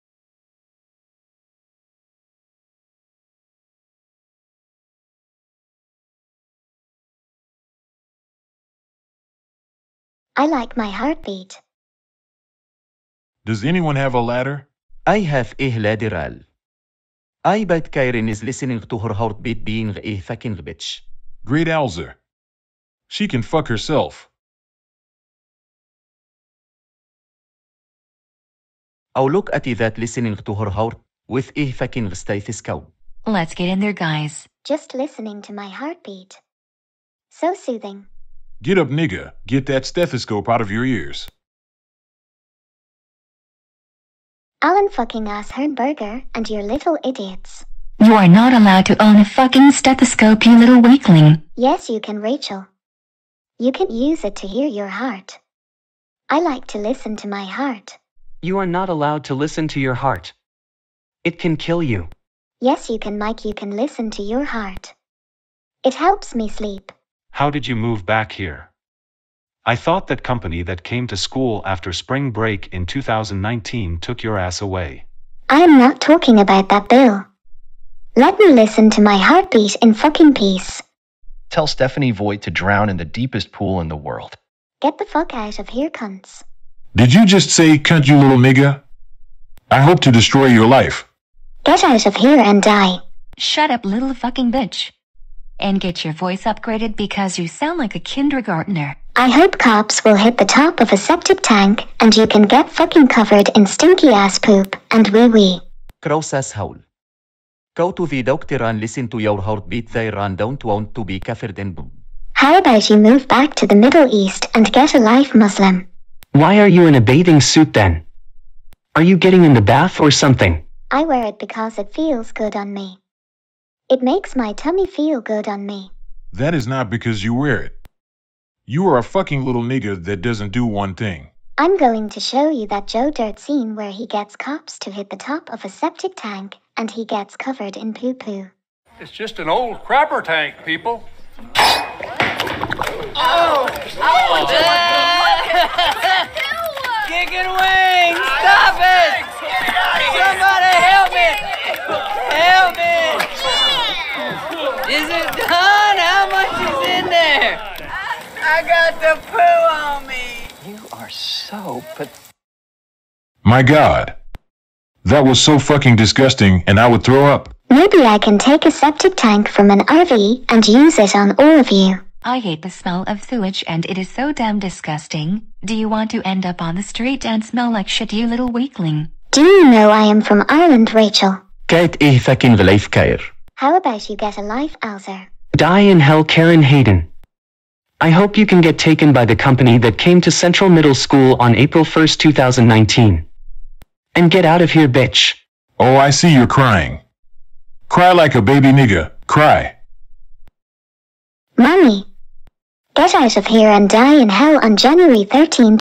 MAKING KAREN HAYDEN CRY HER ASS OFF
Karen hayden owns a stethoscope for no fucking reason when shes doesn't work at a hospital. she is on her ass doing ASMR for no reason and i hate ireland and shit